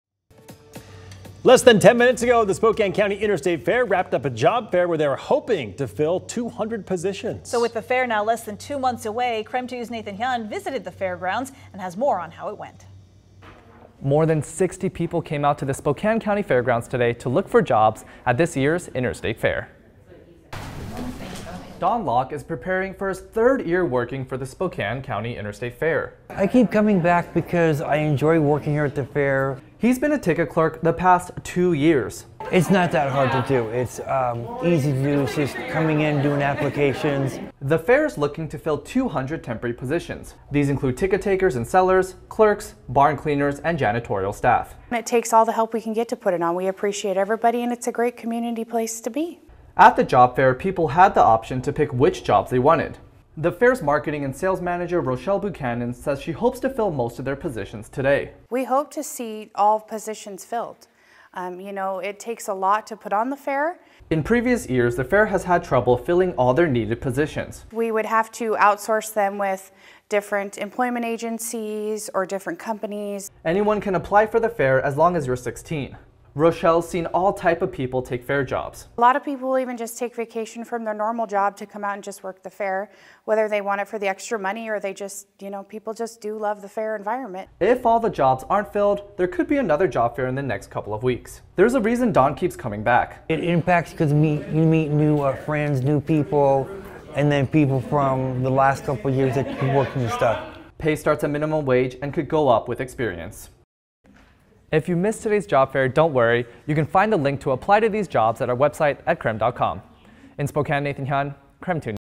Spokane County Interstate Fair hosts job fair in hopes of filling 200 positions
The Interstate Fair is looking to hire more than 200 temporary workers. These include ticket takers and sellers, clerks, barn cleaners and janitorial staff.